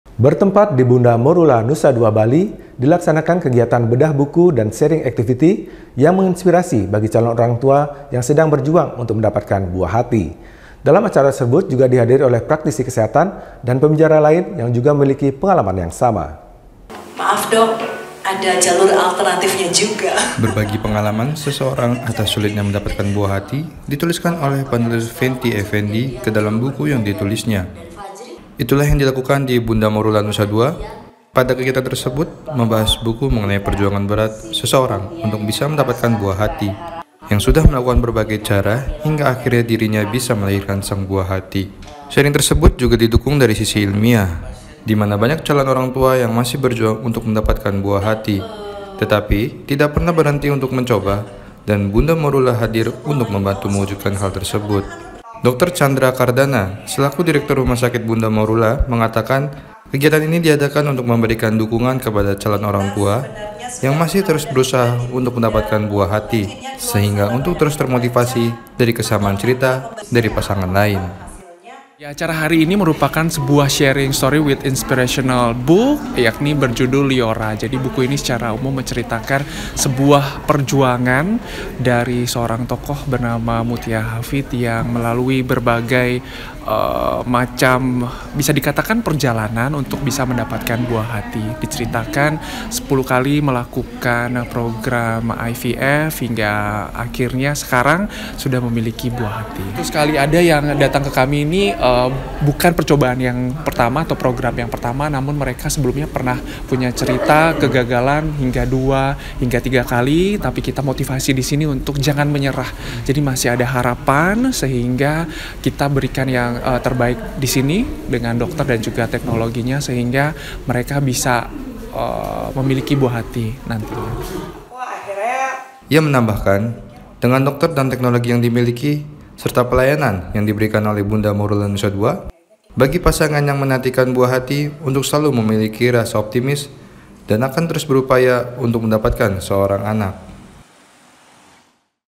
SHARING ACTIVITY BAGI PASANGAN YANG MENANTIKAN BUAH HATI || BERANDA NUSANTARA
BERTEMPAT DI BUNDA MORULA NUSA DUA BALI, DILAKSANAKAN KEGIATAN BEDAH BUKU DAN SHARING ACTIVITY YANG MENGINSIPARI BAGI CALON ORANG TUA YANG SEDANG BERJUANG UNTUK MENDAPATKAN BUAH HATI. DALAM ACARA TERSEBUT JUGA DIHADIRI OLEH PRAKTISI KESEHATAN DAN PEMBICARA LAIN YANG JUGA MEMILIKI PENGALAMAN YANG SAMA.